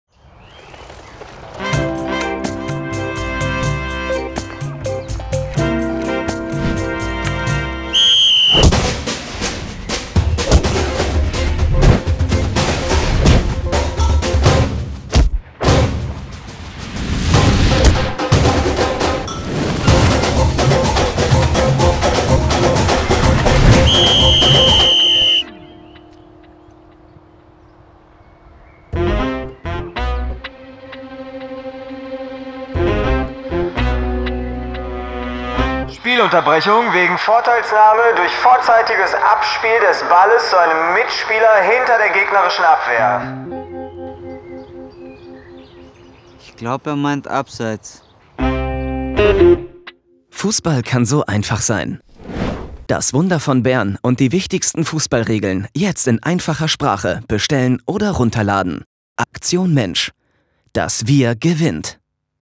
Einfach ist auf'm Platz – "Das Wunder von Bern" in Einfacher Sprache
Hier kannst du dir kostenlos das Hörbuch sowie das eBook von "Das Wunder von Bern" und die Fußballfibel herunterladen – jeweils in Einfacher Sprache und

1. Der Ball ist rund. 2. Das Runde muss in das Eckige. 3. Aber wie war das noch mal mit dem Abseits? Fußball kann so einfach sein: Die wichtigsten Fußballregeln und „Das Wunder von Bern" in Einfacher Sprache.

Rechtzeitig zur Fußball-Weltmeisterschaft in Brasilien wurde mit dem „Wunder von Bern" die Geschichte der WM-Mannschaft von 1954, die Millionen von Zuschauern bewegte, in Einfache Sprache übersetzt. Die Aktion Mensch machte damit gemeinsam mit dem Spaß am Lesen Verlag das Drehbuch von Regisseur Sönke Wortmann einer breiten Bevölkerung zugänglich.

Damit jeder die Weltmeisterschaft mit Interesse verfolgen kann, hat die Aktion Mensch in der Broschüre „Einfach Fußball" die wichtigsten Regeln verständlich erklärt. „Dank weitgehend barrierefreier Bundesliga-Stadien können alle Menschen Fußball live erleben – mit der Fußballfibel können jetzt auch alle mitreden", sagt dazu Rainer Bonhof, Fußball-Weltmeister von 1974.